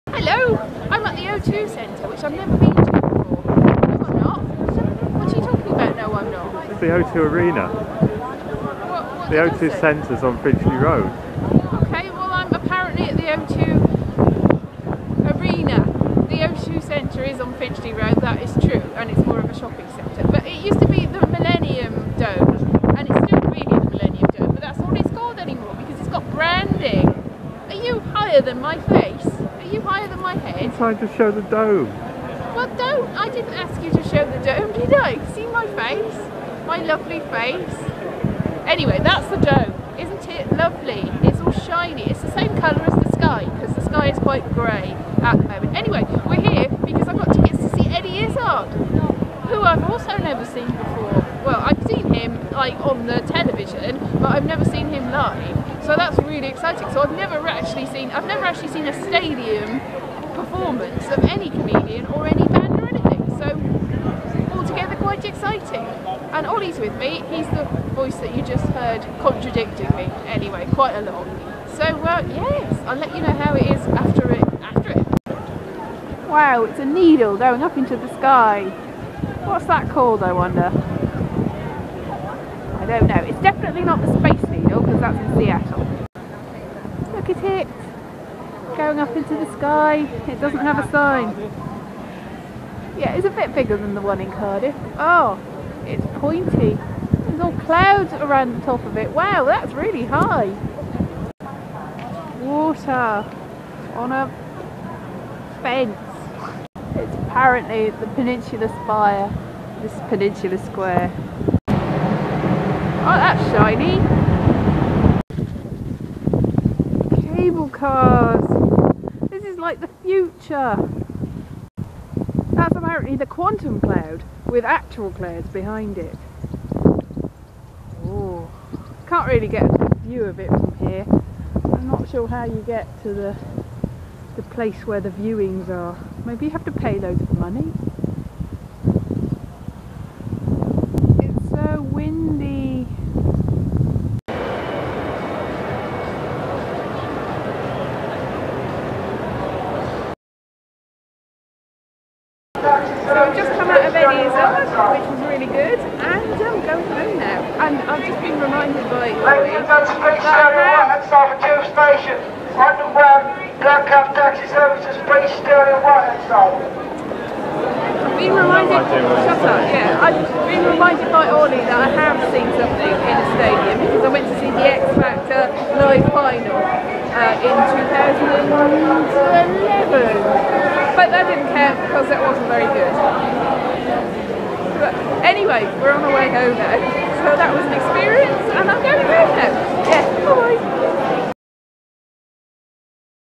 Outside the O2
Went to the O2 Arena (formerly the Millennium Dome) for the first time ever, to see Eddie Izzard live in his 2013 show Force Majeure. Obviously can't show the actual show but here's the dome and surrounding.
It was very windy.